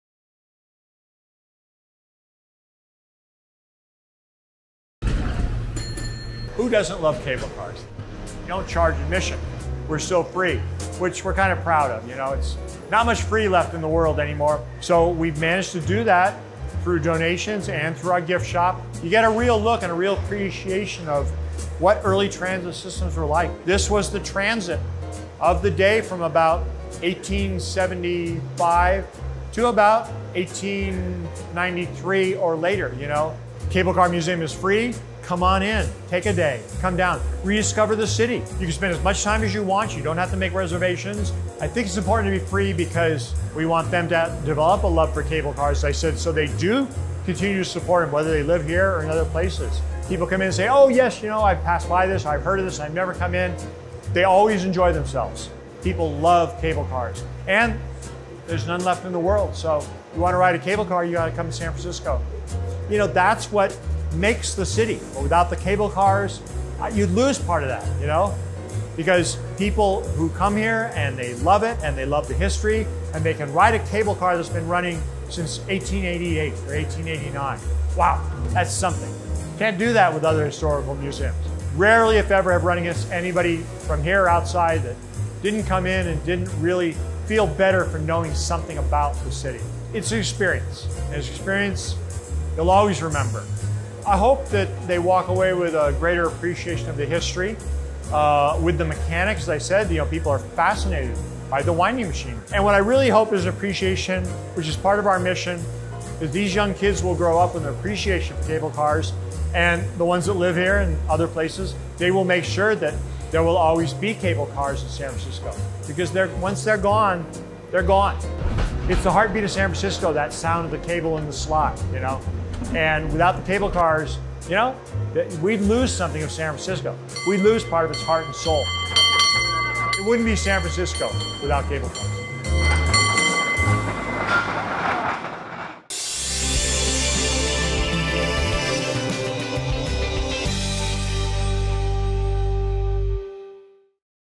The San Francisco Cable Car Museum
Located in the Nob Hill neighborhood at the Washington-Mason powerhouse and car barn, sits a hidden gem, the San Francisco Cable Car Museum. Admission is always free at this popular attraction for visitors and locals alike. The museum houses a collection of historic cable cars, both life size and small, photographs, artifacts and hands on displays. It also features a gift shop run by the Friends of the Cable Car Museum which is a nonprofit organization dedicated to the preservation of cable car history.